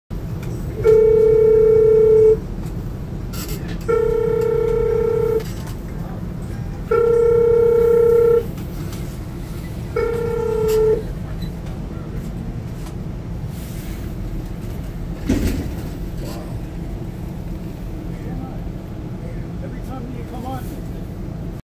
'Zanta' trying to get off a 501 Queen streetcar. (2019)
Zanta trying to get off a streetcar,  (technically he's not allowed on the TTC nor is this kind of scooter, but most of TTC's operators are just so tired of his crap that they don't care anymore.

(ringing is after he flipped out & someone  panicked, he slammed the 'emergency call button with his scooter.)

he is REALLY verbally abusive to TTC operators & anyone who tells him off for being a jerk.